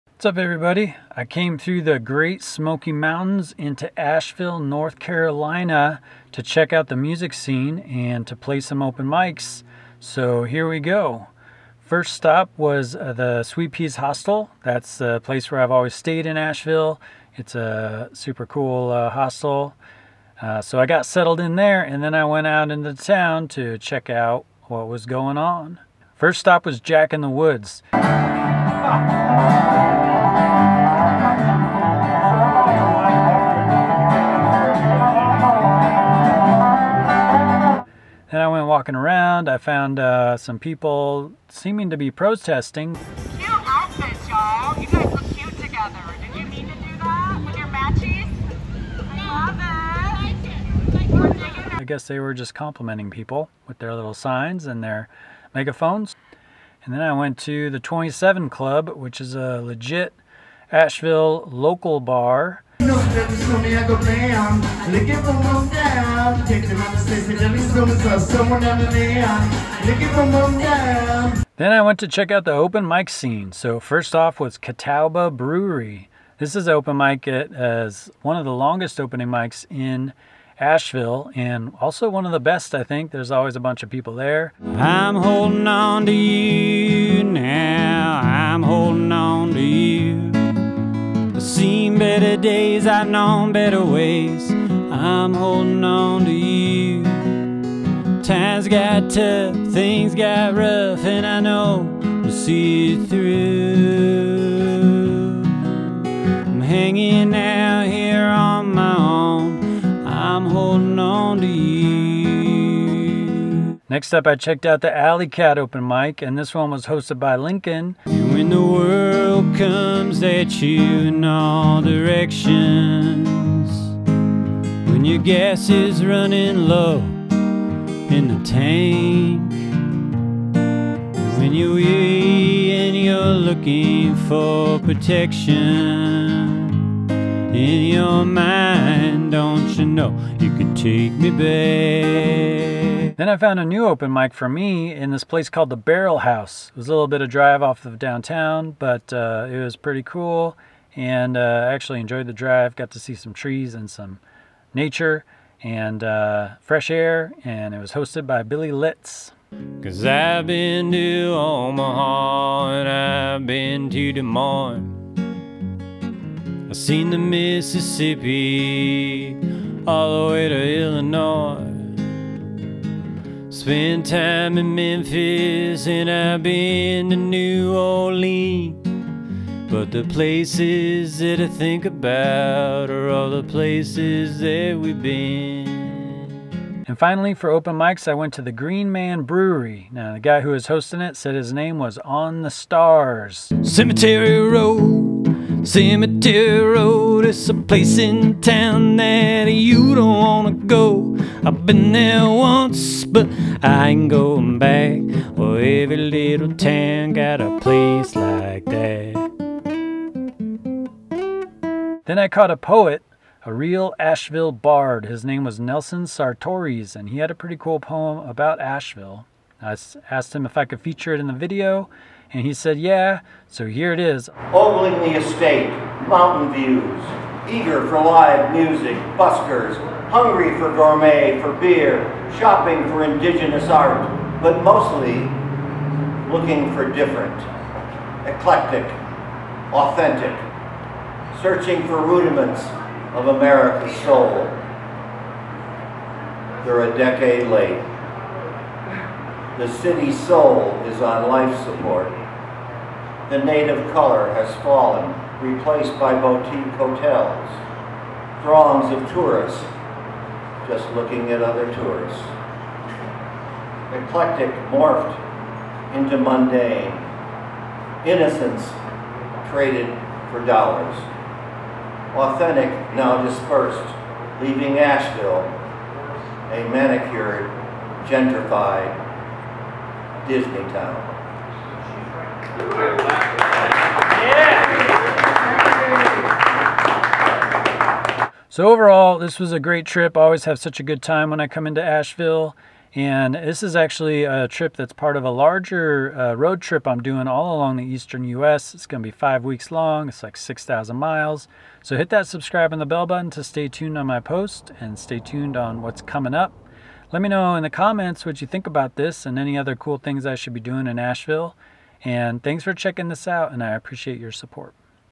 The best of Asheville, North Carolina (Music, breweries, buskers and open mics)
From breweries to buskers and live music, Asheville North Carolina always seems to have something cool going on. This video features the music scene in the city and some other random stuff.

Here's the poet who was featured in the video:
▶Nelson Sartoris